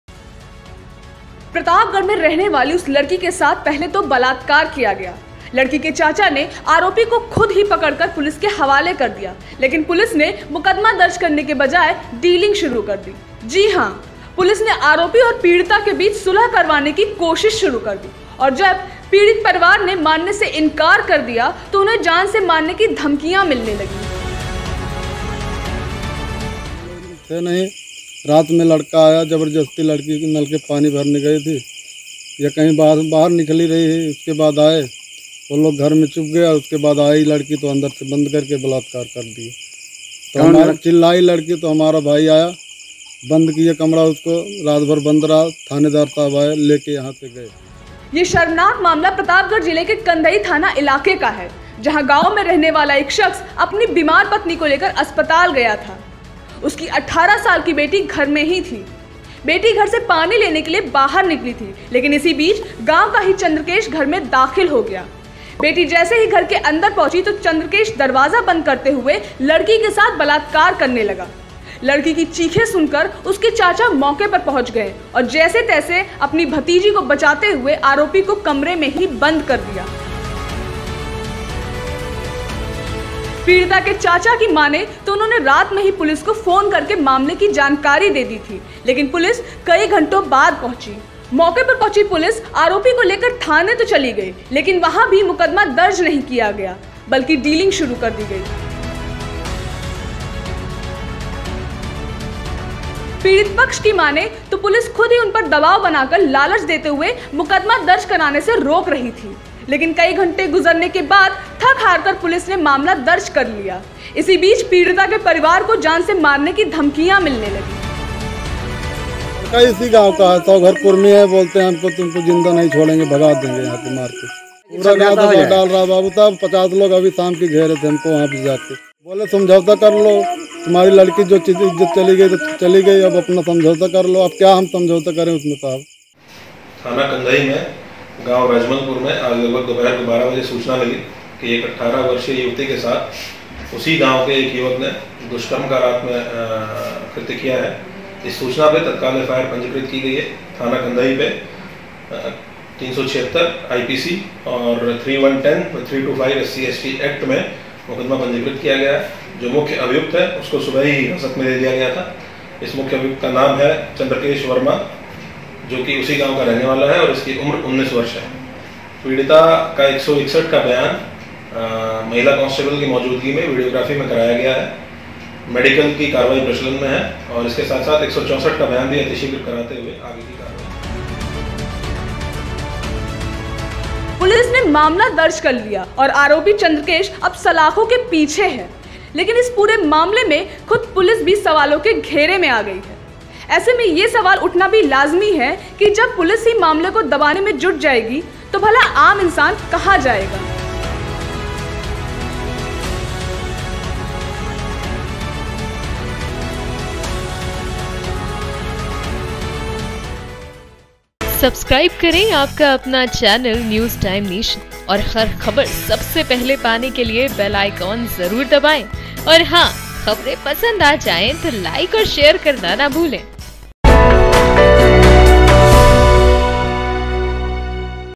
Pratapgarh-बलात्कार के बाद थाने में हुई डील !-| News Time Nation I
प्रतापगढ़ में रहने वाली उस लड़की के साथ पहले तो बलात्कार किया गया...लड़की के चाचा ने आरोपी को खुद पकड़ कर पुलिस के हवाले कर दिया...लेकिन पुलिस ने मुकदमा दर्ज करने के बजाए डीलिंग शुरु कर दी...जी हां पुलिस ने आरोपी और पीड़िता के बीच सुलह करवाने की कोशिशें शुरु कर दी...और जब पीड़ित परिवार ने मानने से इंकार कर दिया तो उन्हें जान से मारने की धमकियां मिलेने लगी...
ये शर्मनाक मामला प्रतापगढ़ जिले के कंधई थाना इलाके का है...जहां गांव में रहने वाला एक सख्स अपनी बीमार पत्नी को लेकर अस्पताल गया था...उनकी 18 साल की बेटी घर में थी...बेटी घर से पानी लेने निकली लेकिन इसी बीच गांव का ही चंद्र केश घर में दाखिल हो गया...बेटी जैसे ही घर के अंदर पहुंची तो चंद्रकेश दरवाजा बंद करते हुए लड़की के साथ बलात्कार करने लगा...लड़की की चीखें सुनकर उसके चाचा मौके पर पहुंच गए और जैसे तैसे भतीजी को बचाते हुए आरोपी को कमरे में ही बंद कर दिया...पीड़िता के चाचा की माने तो उन्होने रात में ही पुलिस को फोन कर मामले की जानकारी दी थी लेकिन पुलिस कई घँटे के बाद मौके पहुंची...
मौके पर पहुंची पुलिस आरोपी को लेकर थाने चली गई लेकिन वहां भी मुकदमा दर्ज नहीं किया गया बल्कि डीलिंग शुरु हो गई...पीड़ित पक्ष की माने को पुलिस खुद ही उनपर दबाव बना कर लालच देते हुए मुकदमा दर्ज कराने से रोक रही थी...कई घंटे गुजरने के बाद थक हार कर पुलिस ने मामला दर्ज किया...लेकिन इसी बीच पीडिता के परिवार को भी जान से मारने की धमकियां मिलने लगी है...